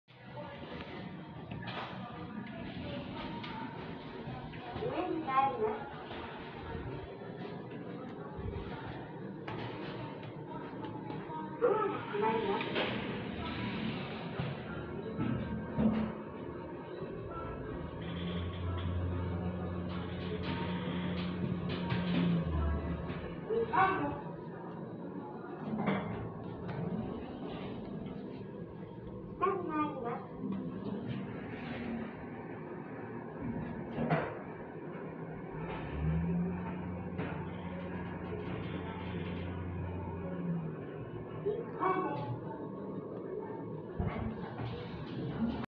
東芝エレベーター
吉野川ハイウェイオアシスにあります。機種はスペーセルです。
メーカー:東芝
設置年:2000年
定員:15名
積載:1000kg
キャンセル機能:あり
アナウンス:あり
戸閉ブザー:なし
到着ベル音:なし
ボタン確認音:なし
停止階:1.2
停止可能階:1.2